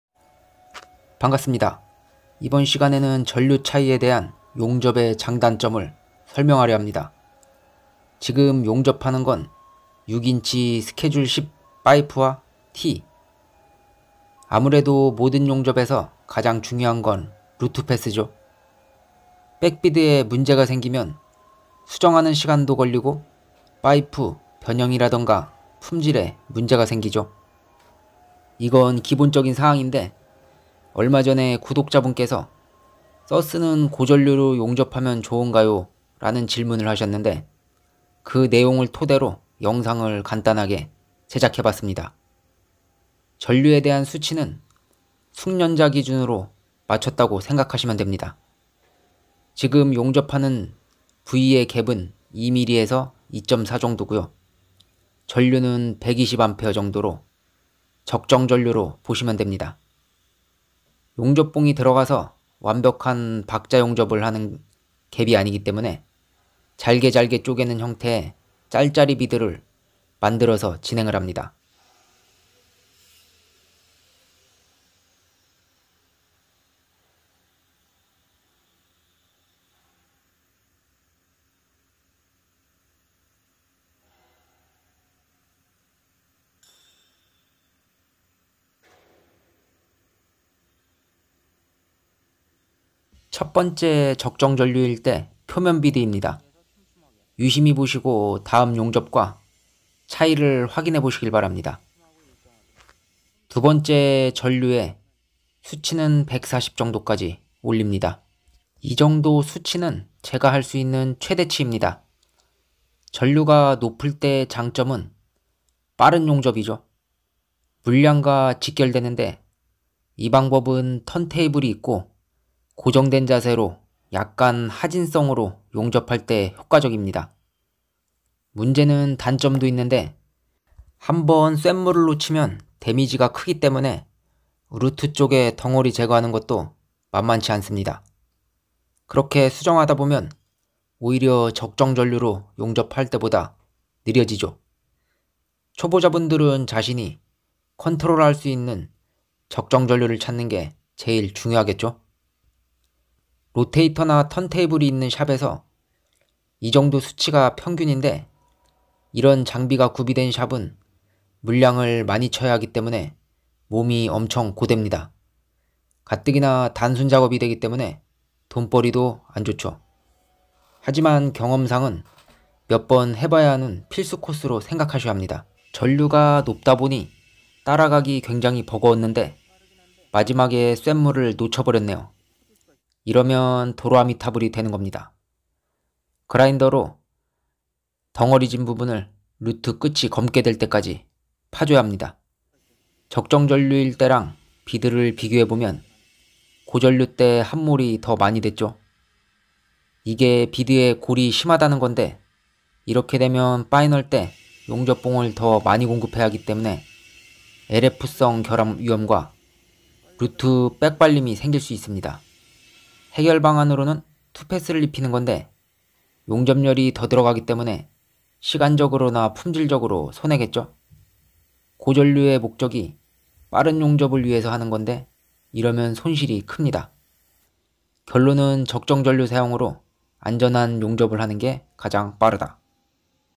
지금까지 이런 분석은 없었다 (용접전류 차이점)
안녕하세요
오늘은 용접 전류의 차이점을 알아 봅니다
(본영상은 회원 영상으로 시간이 지나 게시됨을 알려 드립니다 )